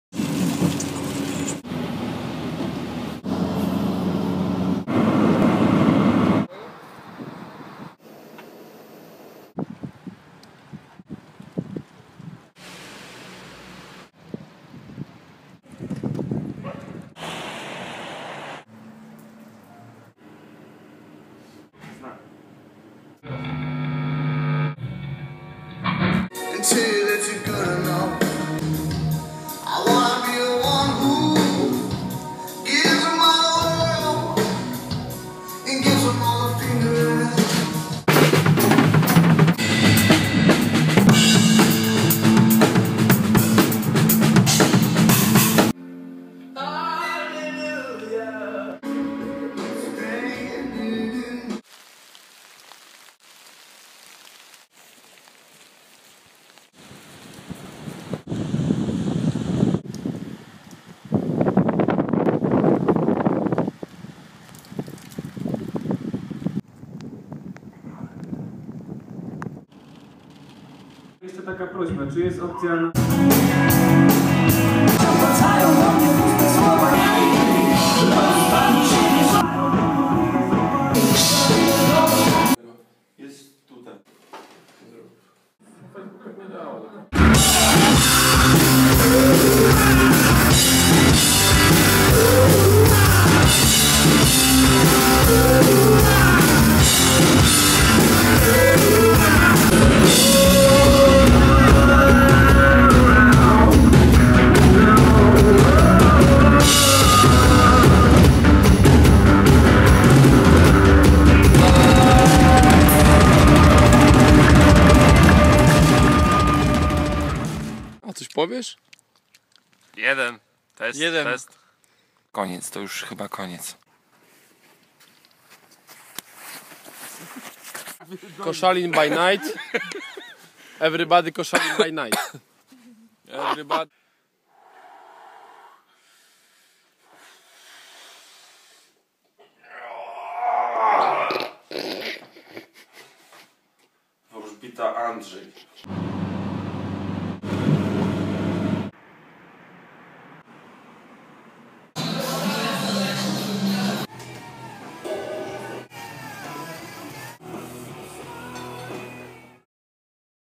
Cuba de Zoo - Sopot, Koszalin, Międzyrzecz 19-20-21.11.2015
Cuba de Zoo - Trasa Jesień 2015
Odcinek Pomorze III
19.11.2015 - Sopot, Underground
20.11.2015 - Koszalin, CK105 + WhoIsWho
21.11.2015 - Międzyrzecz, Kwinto